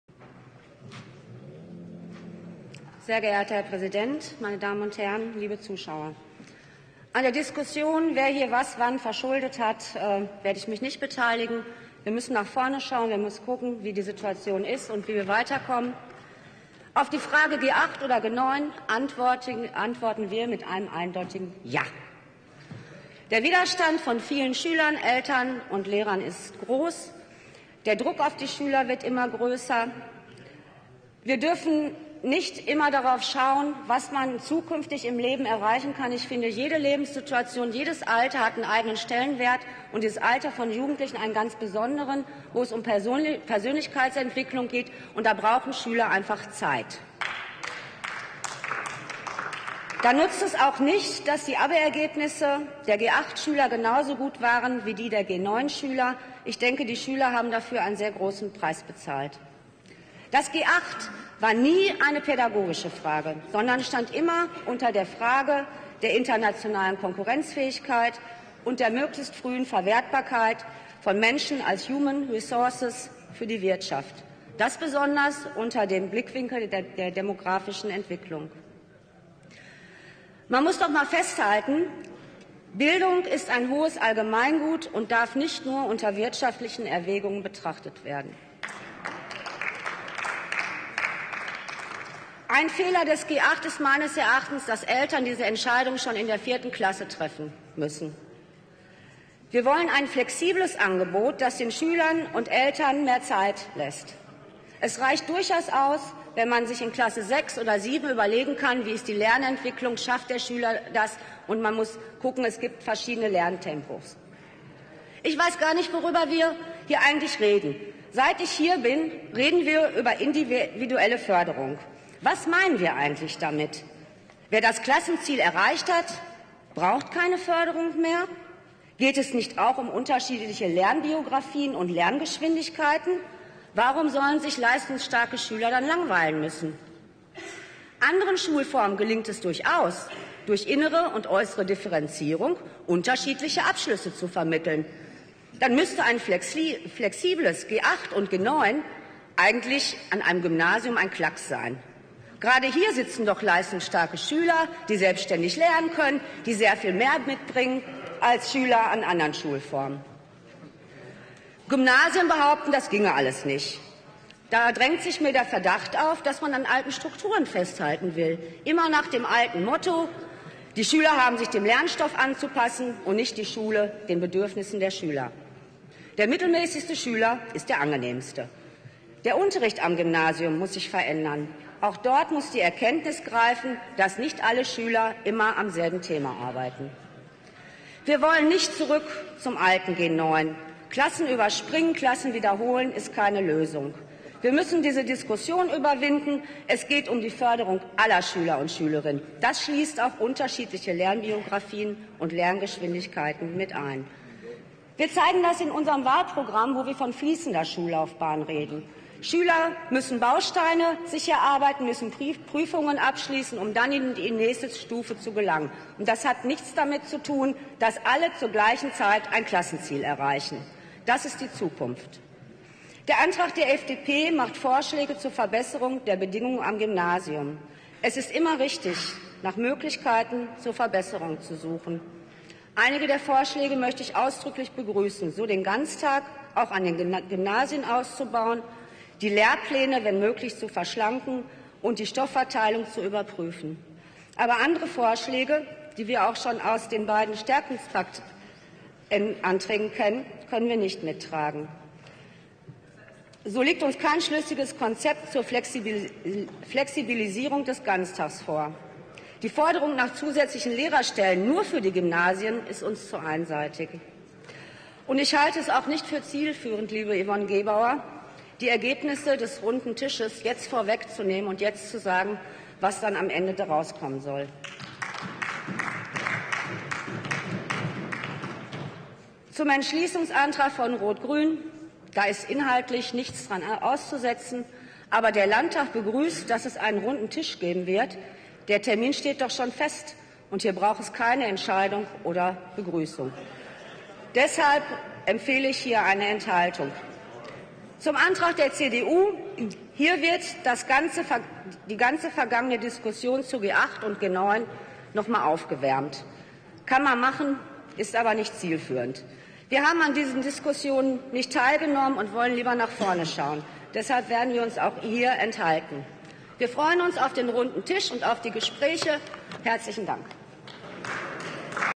Monika Pieper (@monipiratin) zum Qualitätserhalt in Gymnasien statt Strukturdebatten
Top 3. Kein Chaos in die Gymnasien tragen -- innere Qualität stärken statt große Unruhe durch erneute Strukturdebatten herbeiführen -- Wahlmöglichkeiten erhalten
Drucksache 16/5472
direkte   Abstimmung
Abstimmungsempfehlung: Ablehnung